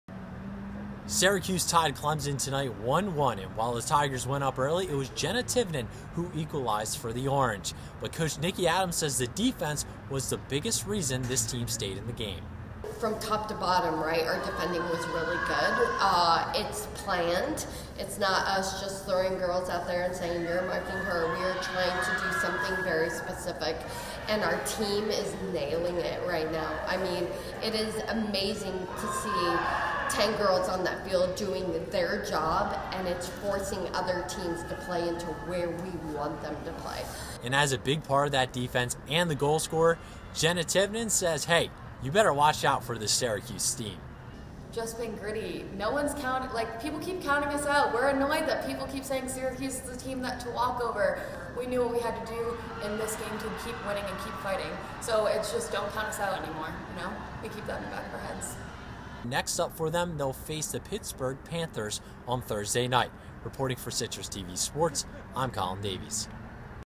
Syracuse Women's Soccer Draws Clemson with Great Defense
Syracuse tied Clemson 1-1. This game was a 180 from the last year when the Orange lost 8-0 to the Tigers. Coach Nicky Adams attributes the win to defense.